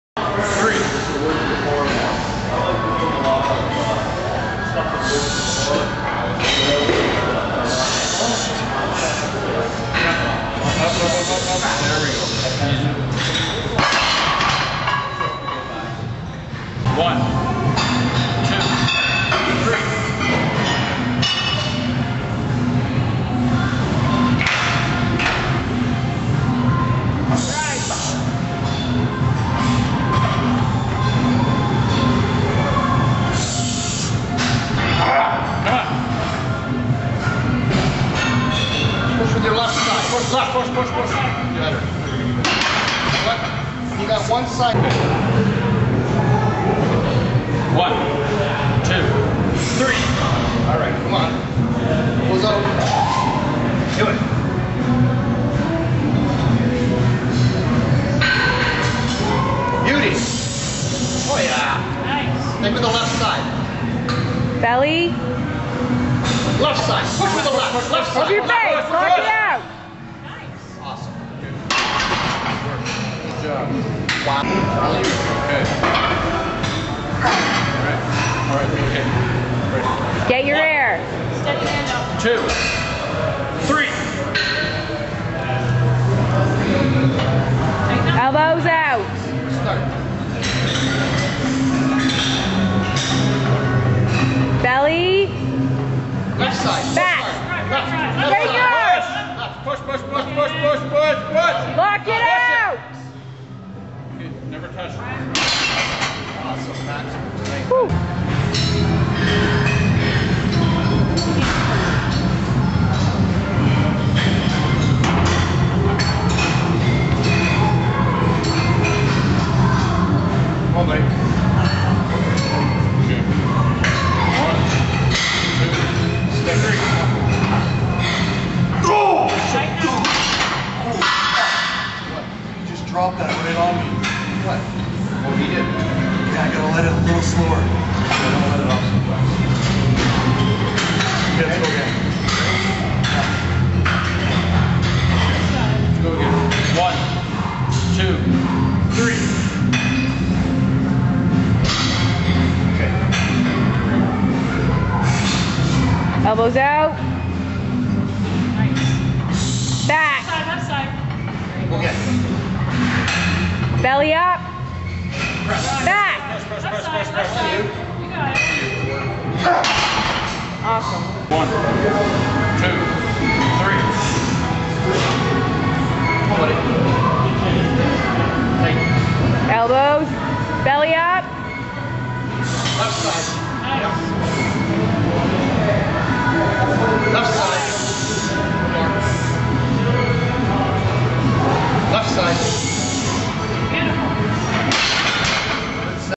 The Crooked Bencher.wmv
this is a big problem Ive been having lately. My right shoulder wont pull back as far as my left. I feel flat and balanced but obviously Im not. The Crooked Bencher.